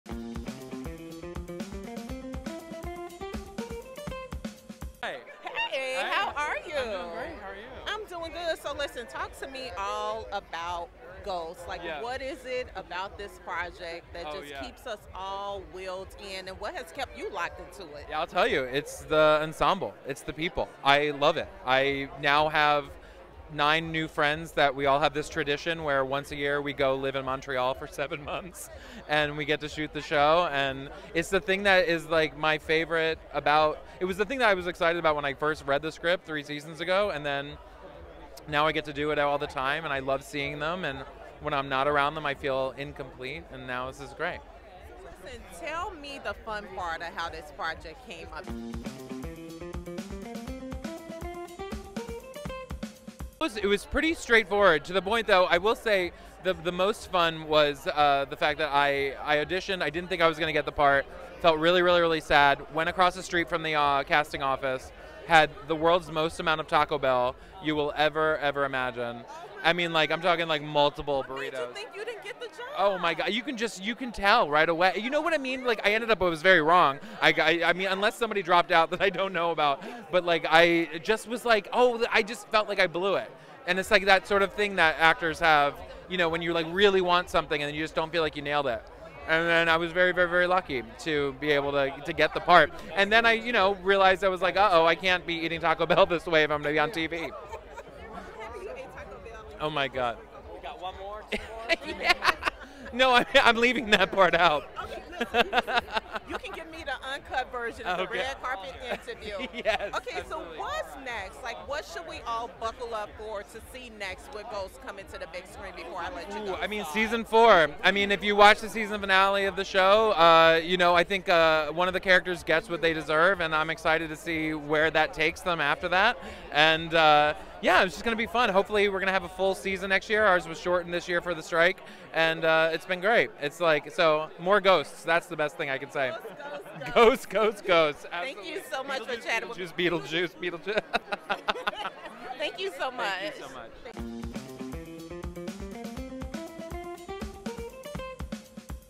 Brandon Scott Jones ('Ghosts') at 2024 CBS New Fall Schedule Party red carpet
The CBS New Fall Schedule Party took place Thursday, May 2 at Paramount Studios in Hollywood. Network stars, producers and executives were in attendance to celebrate the Fall 2024 lineup. Gold Derby's Latasha Ford, Marcus James Dixon and Jaclyn Ben-Porat were on the red carpet to interview many of the attendees.